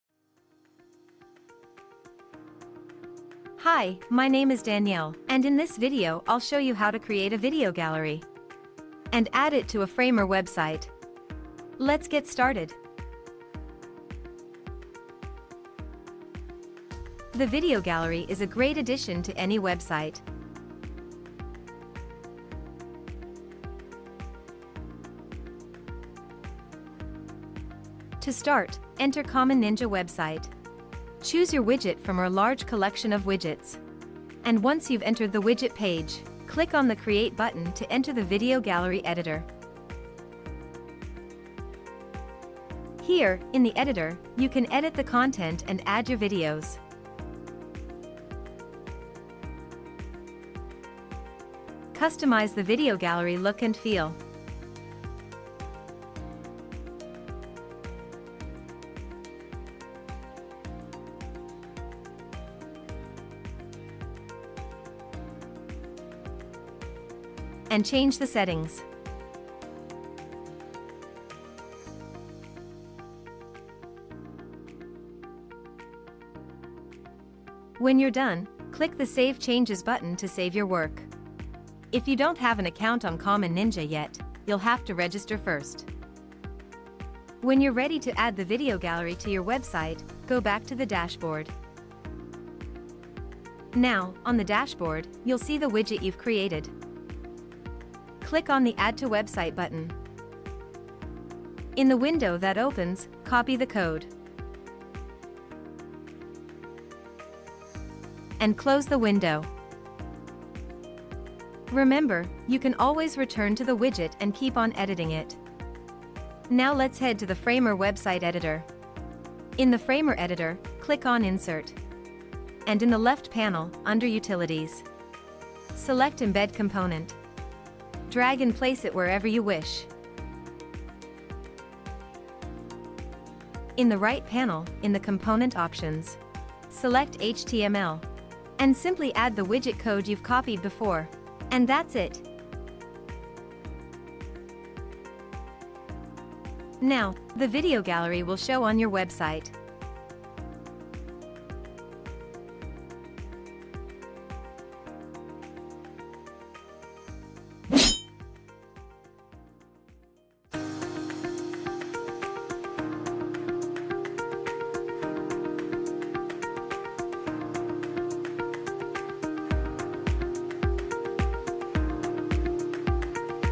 How to add a Video Gallery to Framer
Common Ninja’s Video Gallery widget is a visually appealing tool that will enable you to add videos to your website in the form of a beautiful gallery. With it, you can promote your services or products in the form of videos, add educational videos to help users, enrich your content, and much more!

Video Timeline
00:00 About the Video Gallery widget
00:27 Creating Video Gallery widget
00:37 Editing the Video Gallery widget
01:31 Adding the Video Gallery widget to Framer

The Video Gallery widget for Framer has a wide variety of customizable features, including:
- Playback options
- Captions and descriptions
- Multiple video sources
- Fully customizable